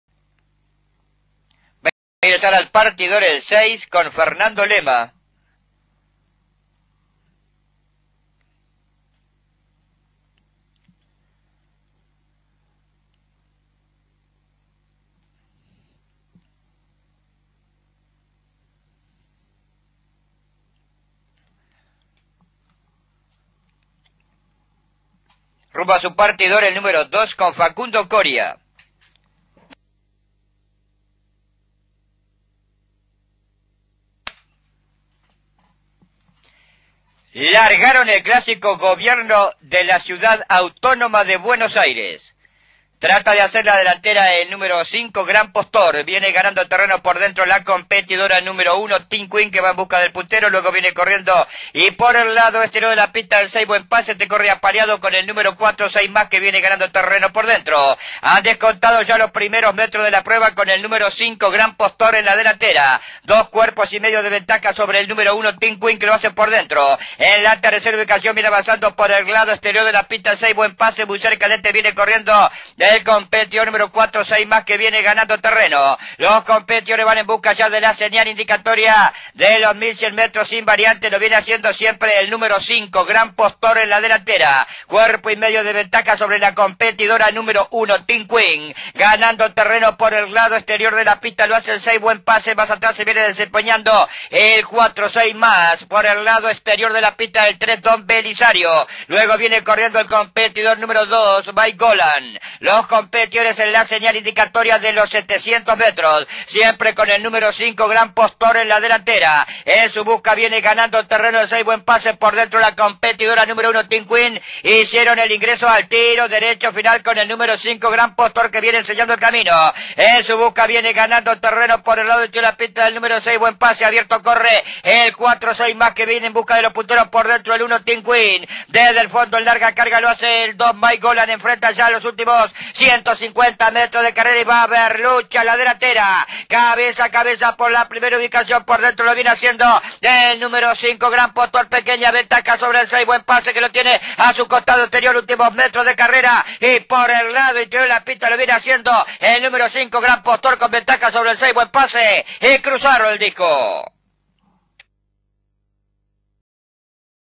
Gran Postor Clásico Gobierno de la Ciudad de Buenos Aires 2009 (L) Hipódromo Palermo
Gran Postor (Big Bidder - Rubia Mamina) venció en el Clásico Gobierno De La Ciudad Autónoma de Buenos Aires (L) disputado el lunes 07/12/09 en el Hipódromo de Palermo.

El conducido por Juan Carlos Noriega se impuso de punta a punta, guapeando y luchando de los 400 a la raya cuando Buen Pase vino a ganarle. Cruzó el disco con ½ cpo de ventaja sobre su rival. 3º, a 5 cps del escolta, llegó Sei Mas, que nunca fue de peligro.

El hijo de Big Bidder, al cuidado de Juan Carlos Bianchi y defendiendo los colores del stud Mamina empleó un tiempo de 1'48"42 para recorrer los 1800 mts del Clásico, sobre una pista de arena normal con viento en contra en la recta principal.

Así obtuvo su 4ª victoria en 19 salidas a la cancha y 1ª  jerárquica. Ganó sus 4 carreras en pista de arena: 2 en el hipódromo de Palermo y otras 2 en el hipódromo de La Plata. Se impuso en 4 distancias distintas: 1400, 1600, 1700 y 1800 mts. Cotizado en 4º término en las apuestas pagó a ganador 3,60.

El hijo de Rubia Mamina (Gold Trojan) de pelaje zaino negro, nació el 06/08/2005 en el Haras Alborada.

Campaña: 19 Carr: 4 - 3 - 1, $ 173.000